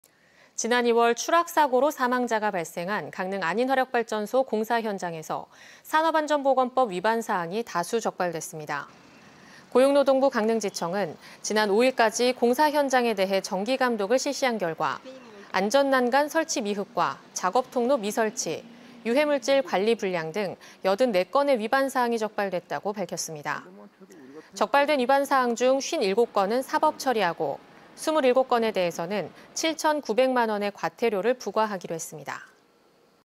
[단신] 강릉 안인화력 산업법 위반사항 84건 적발 210315
지난 2월 추락사고로 사망자가 발생한
강릉안인화력발전소 공사현장에서
산업안전보건법 위반사항이 다수 적발됐습니다.
           ◀ＥＮＤ▶
 고용노동부 강릉지청은 지난 5일까지
공사현장에 대해 정기감독을 실시한 결과
안전난간 설치 미흡과 작업통로 미설치,
유해 물질 관리 불량 등 84건의 위반사항이
적발됐다고 밝혔습니다.

 적발된 위반사항 중 57건은 사법처리하고,
27건에 대해서는 7천9백만 원의 과태료를 부과하기로 했습니다.//